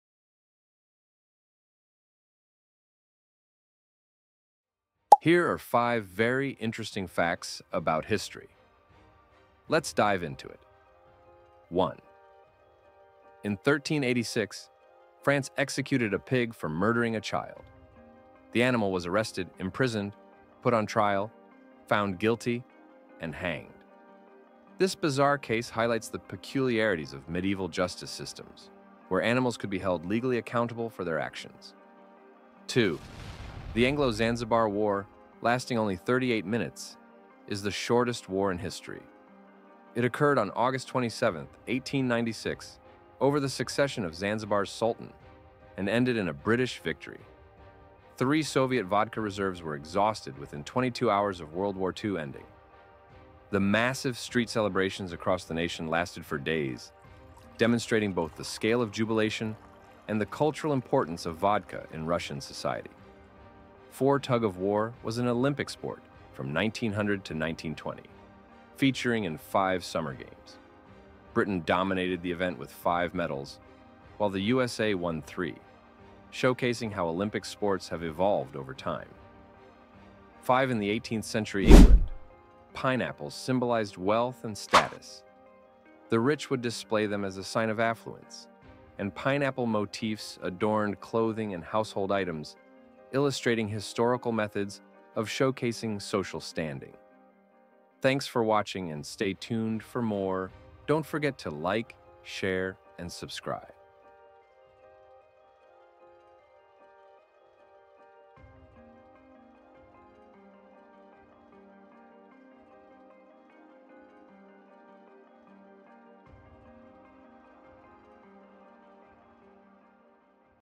Very Interesting History Facts!!
From 0:04 - 1:39 Starting
From 1:40 - 2:01 End Screen

Here are 5 Very interesting Facts About History Let's dive in to it:-

1-In 1386, France executed a pig for murdering a child. The animal was arrested, imprisoned, put on trial, found guilty, and hanged. This bizarre case highlights the peculiarities of medieval justice systems, where animals could be held legally accountable for their actions.

2-The Anglo-Zanzibar War, lasting only 38 minutes, is the shortest war in history. It occurred on August 27, 1896, over the succession of Zanzibar's Sultan and ended in a British victory.

3-Soviet vodka reserves were exhausted within 22 hours of World War II ending. The massive street celebrations across the nation lasted for days, demonstrating both the scale of jubilation and the cultural importance of vodka in Russian society.

4-Tug of War was an Olympic sport from 1900 to 1920, featuring in five Summer Games. Britain dominated the event with five medals, while the USA won three, showcasing how Olympic sports have evolved over time.

5-In 18th century England, pineapples symbolized wealth and status. The rich would display them as a sign of affluence, and pineapple motifs adorned clothing and household items, illustrating historical methods of showcasing social standing.